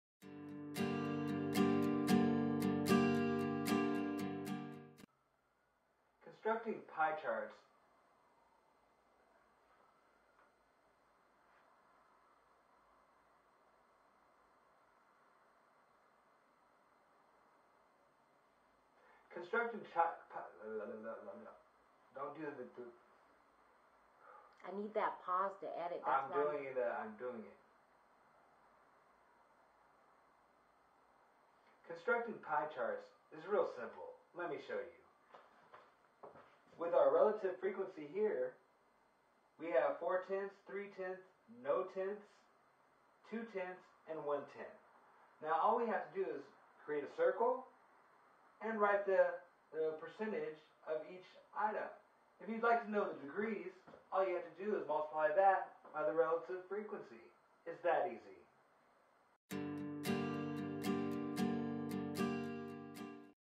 Pie Charts
Math Geeks Tutorial: Pie Charts

Learn how to create Pie Charts to manage Data in Statistics.